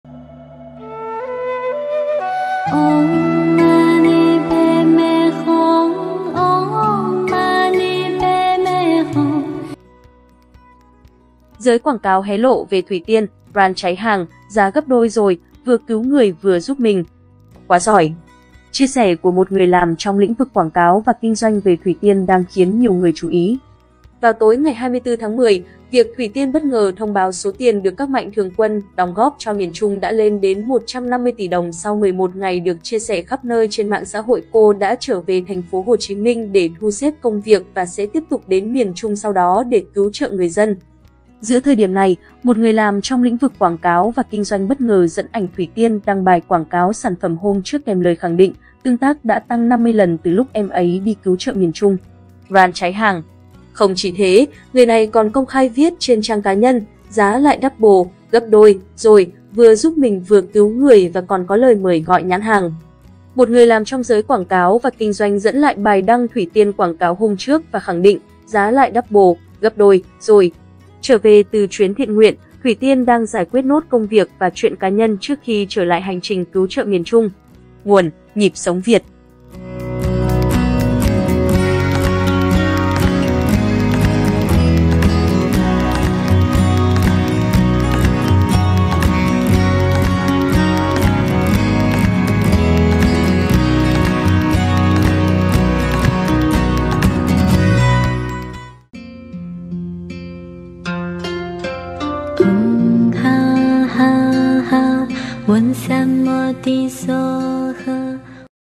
Góc nhân quả | Giá quảng cáo của Thủy Tiên tăng gấp đôi sau chuyến từ thiện ở Miền Trung
Giữa thời điểm này, một người làm trong lĩnh vực quảng cáo và kinh doanh bất ngờ dẫn ảnh Thủy Tiên đăng bài quảng cáo sản phẩm hôm trước kèm lời khẳng định: "Tương tác đã tăng 50 lần từ lúc em ấy đi cứu trợ miền Trung. Brand cháy hàng". Không chỉ thế, người này còn công khai viết trên trang cá nhân: "Giá lại double (gấp đôi) rồi, vừa giúp mình vừa cứu người" và còn có lời mời gọi nhãn hàng.
Đám cưới Thủy Tiên - Công Vinh đãi tiệc chay | Không muốn sát sinh trong ngày cưới